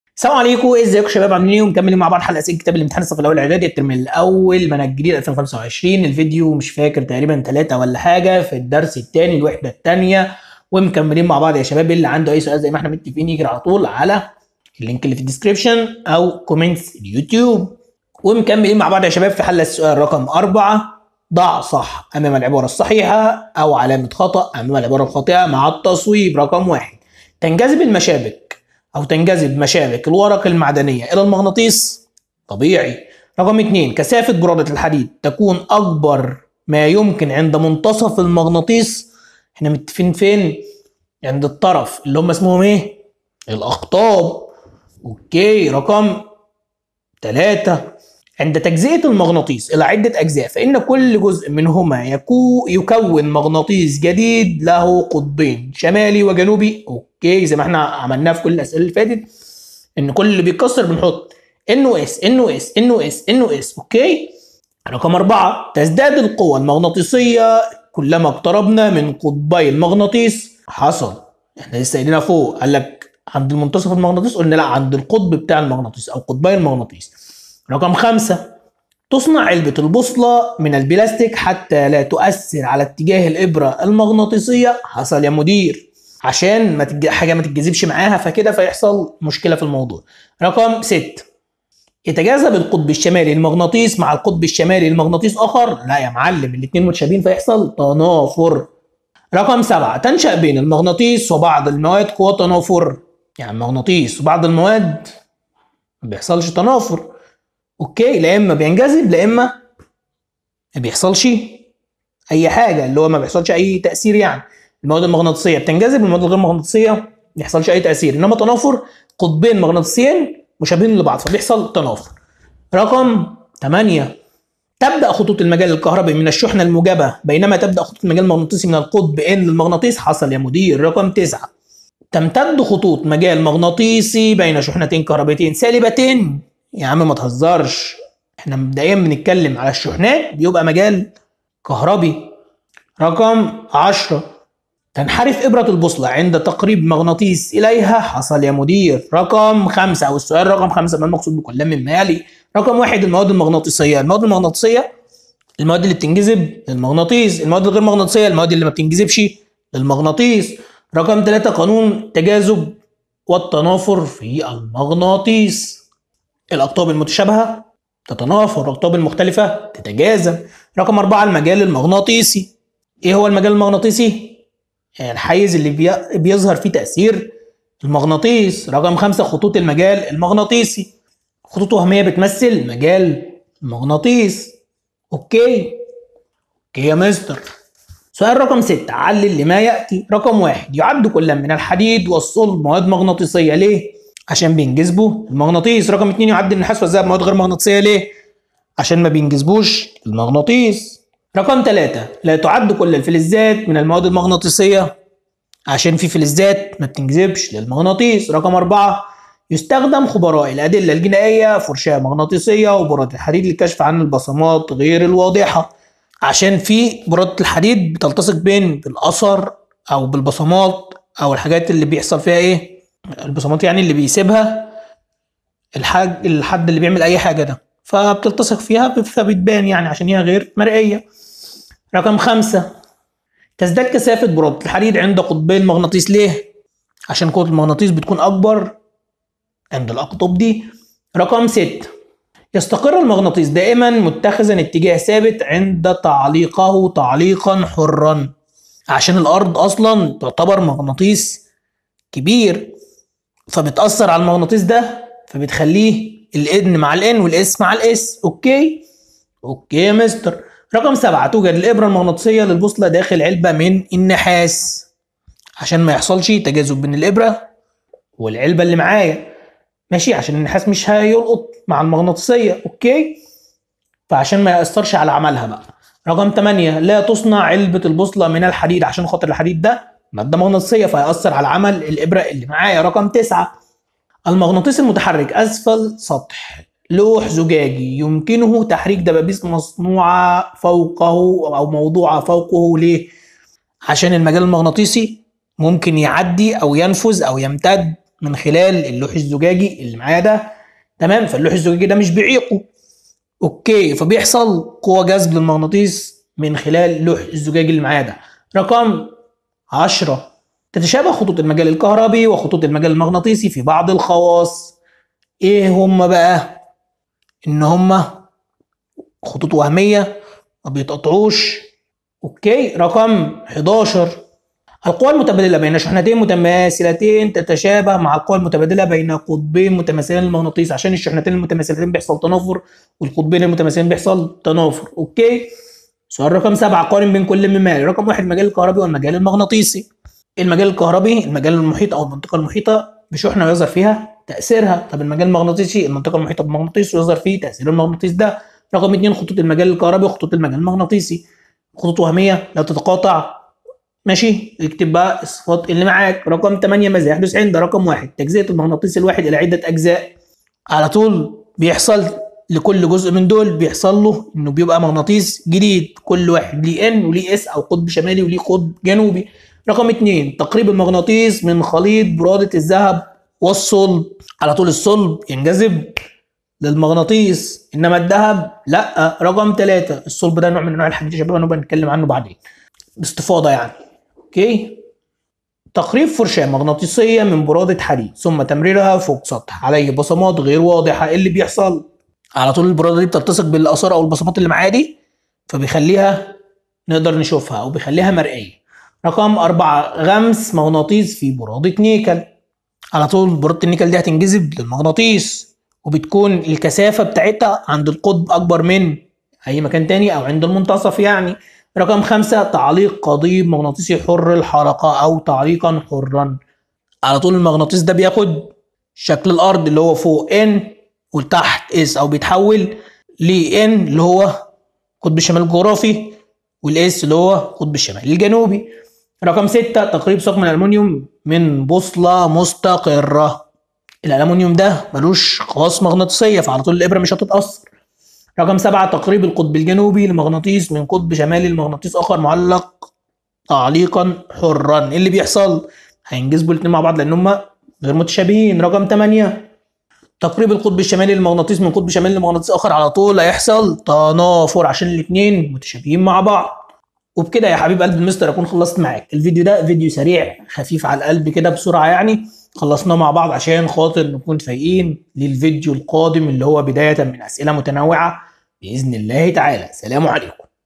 حل اسئلة كتاب الامتحان علوم الصف الاول اعدادي الترم الاول 2025 المنهج الجديد الوحدة 2 الدرس 2 حلقة 3
دا لينك التواصل
Mr / Mustafa Bandery
علوم الصف الاول الاعدادي المنهج الجديد 2025 الوحدة الثانية مجالات القوي الدرس الثاني القوي المغناطيسية